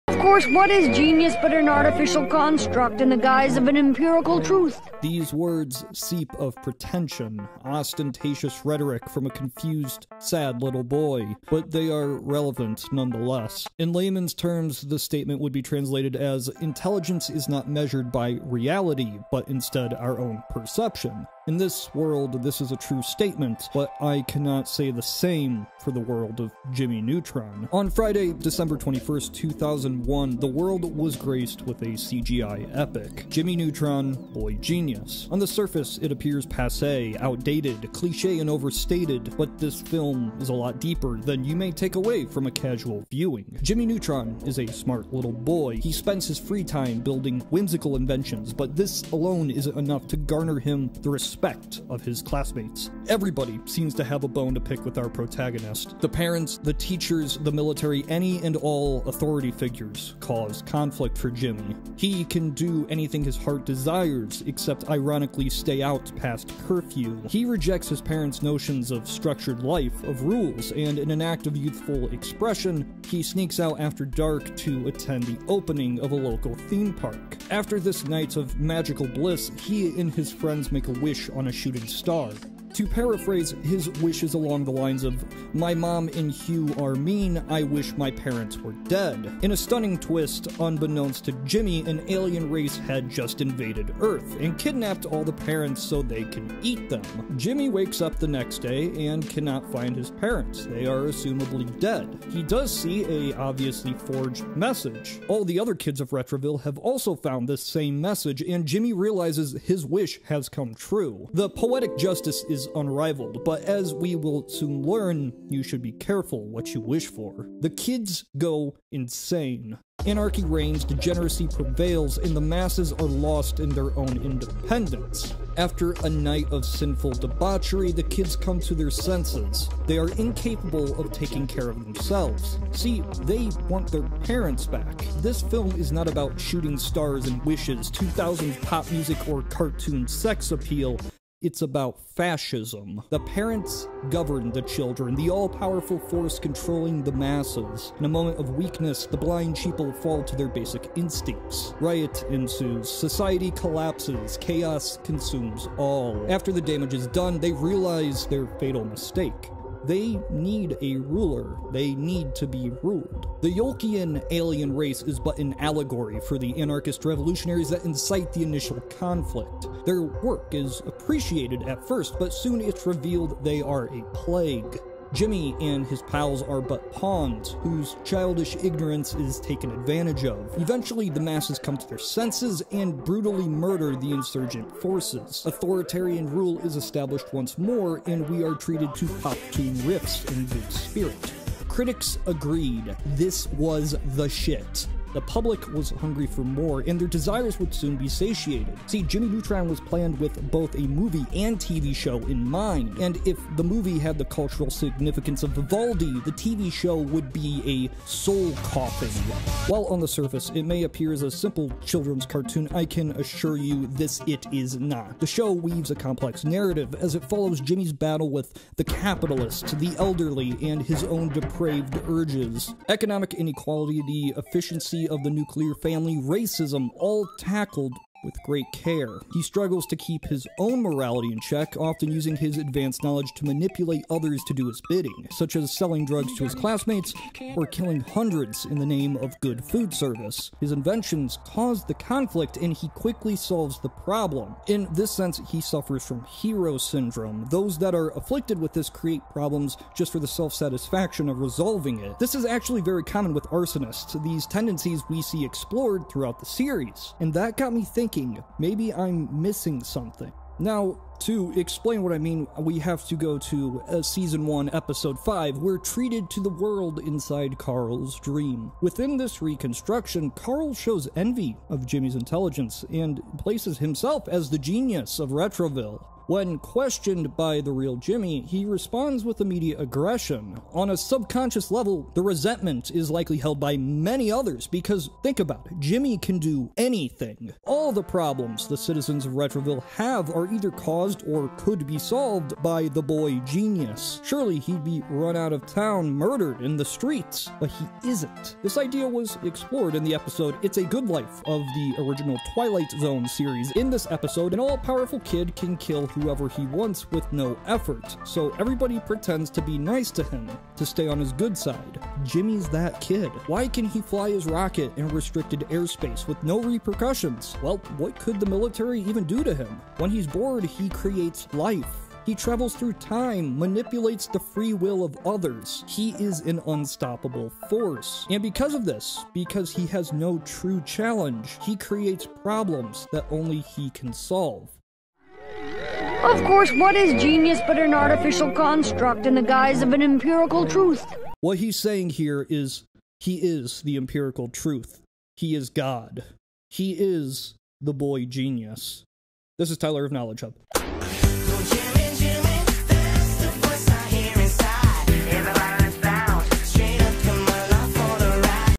An Intellectual Look At Jimmy Neutron
I explain the underlying motifs of the Jimmy Neutron franchise. Get ready for a brain blast.